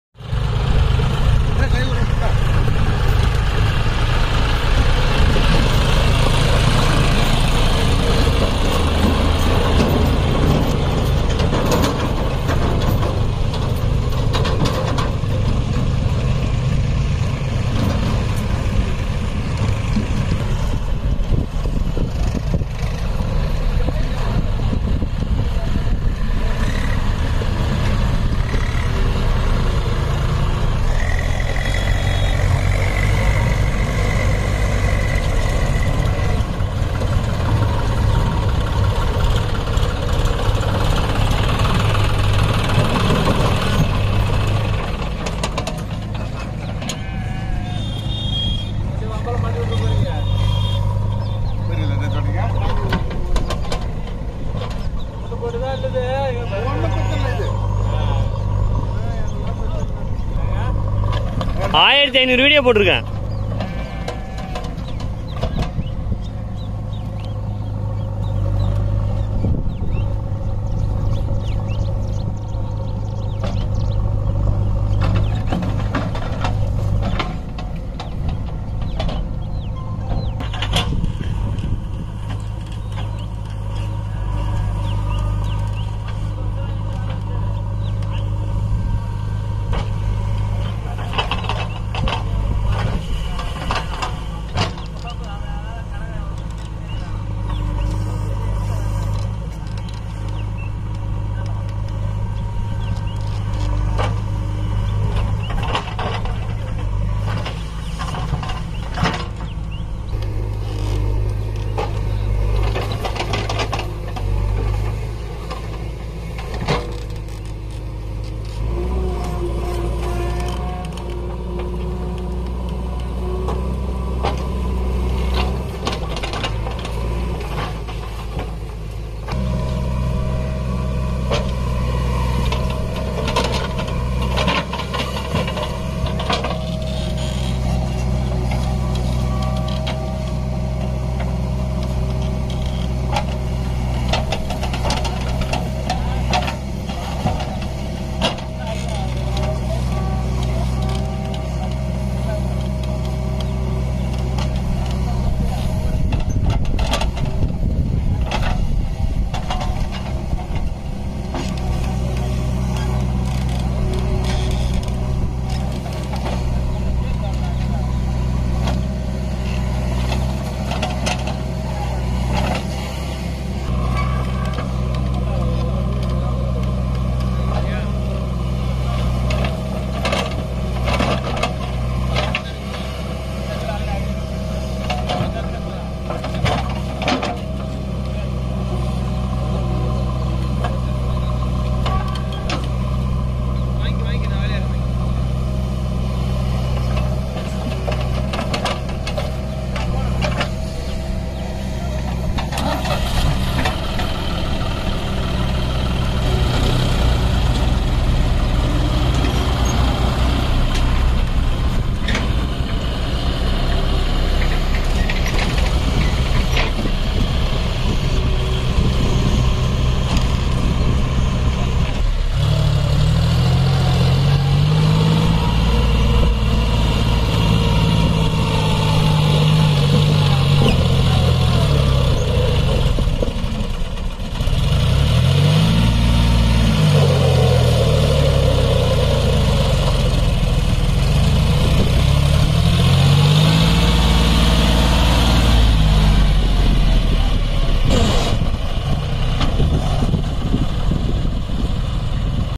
JCP and Tractor videos | John Deere 5050 D Tractor fully loaded by JCB 3DX machine
JCP and Tractor doing mud transportation work in the village. John Deere, tractor 5045 D comes and fully mud loaded by JCB 3DX machine in the village. After fully loaded then the tractor transport the mud to another agricultural land.

John Deere 5045D Specifications:

Engine
Cooling Coolant - Cooled with overflow reservoir
Air Filter - Dry-type, Dual Element

Transmission
Type - Collarshift
Clutch - Single / Dual
Alternator - 12 V 40 A
Forward Speed - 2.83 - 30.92 kmph
Reverse Speed - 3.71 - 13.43 kmph

Brakes
Brakes - Oil Immersed Disc Brakes

Steering
Type - Power

Power Take-Off
Type -Independent, 6 Spline
RPM - 540@1600/2100 ERPM

Turning Radius With Brakes - 2900 MM

Hydraulics
Lifting Capacity - 1600 Kgf
3 point Linkage - Automatic depth and Draft Control

Wheel Drive - 2 WD
Front - 6.00 x 16
Rear - 13.6 x 28

Others Information
Accessories Ballast Weight, Hitch, Canopy
Options RPTO, Adjustable Front Axle, Adjustable Seat
Additional Features - Collarshift type gearbox, Finger guard, PTO NSS, Water separator, Underhood exhaust muffler
Warranty - 5000 Hours/ 5 Yr
Status - Launched